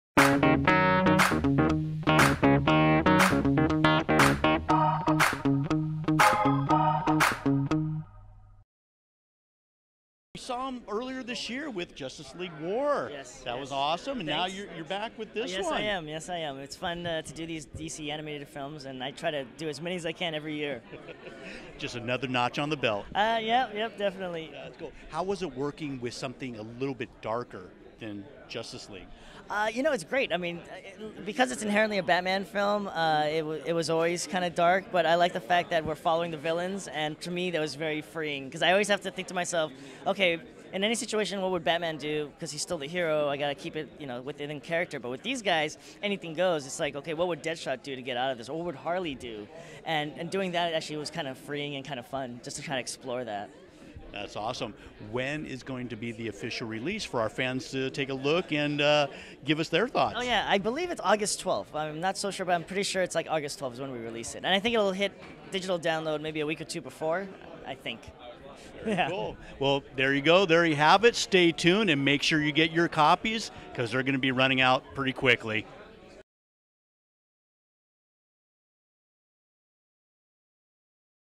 That's My Entertainment Interviews Jay Oliva from Batman Assault on Arkham
Warner Brothers Animation is releasing a new Batman animated film called Batman: Assault on Arkham.  Director Jay Oliva talks about this humbling experience and what's in store for the fans!

That's My Entertainment! What's Yours?